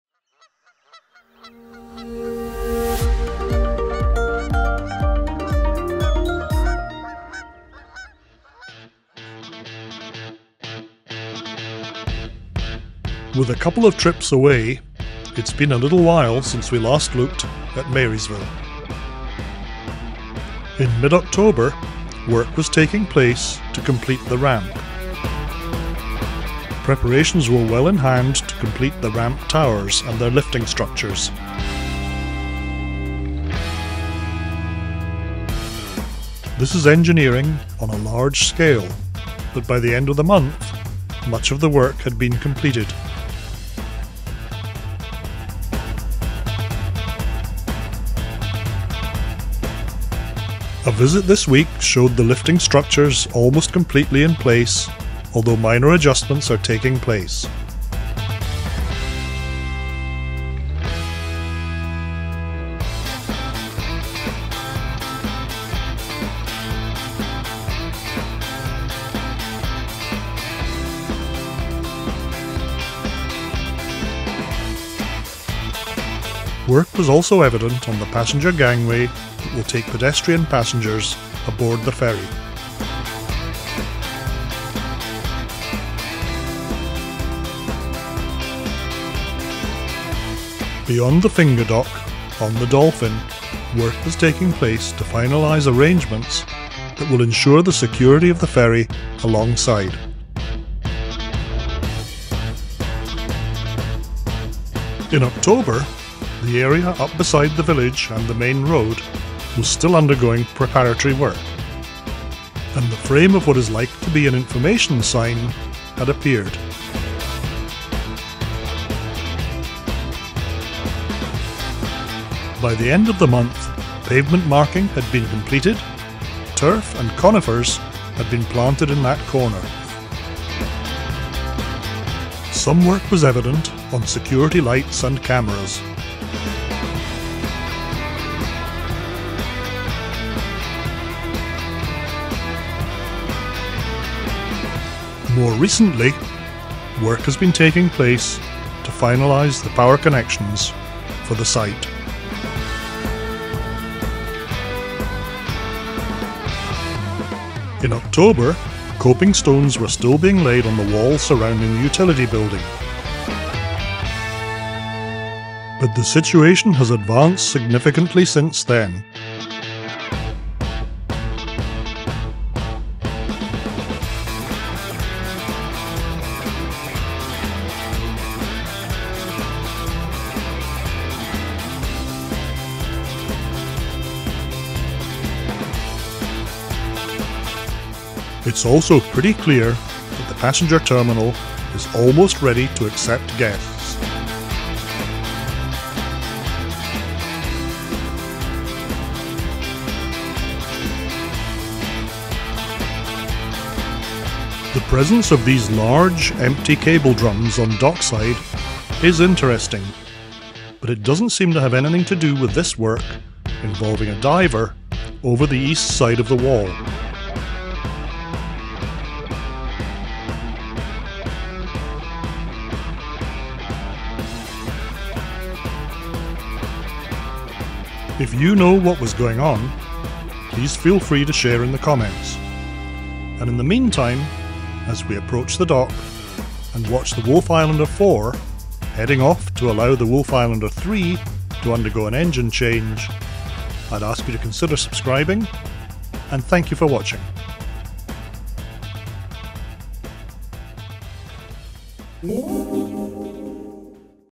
Can we see the Finish Line - an update on the Marysville Dock 4K
This update, completed in the first week of December catches up with the state of play in Marysville.  It seems clear that the completion of work on this dock is very close. How close is anyone's guess, but the work done here has been consistently good.  Look out for future updates to see how it all turns out.

Chapters:
00:12 - The Main Ramp and Lift Mechanisms
01:14 - Passenger Gangway and Mooring Arrangements
01:44 - SE Corner and Distribution Equipment
02:37 - Utility Building and Passenger Terminal
03:23 - Work over the side - dive operations

Aerosnapper is a proud member of the Drone Pilot Association of Canada - If you fly drones and you’re not already a member, please consider joining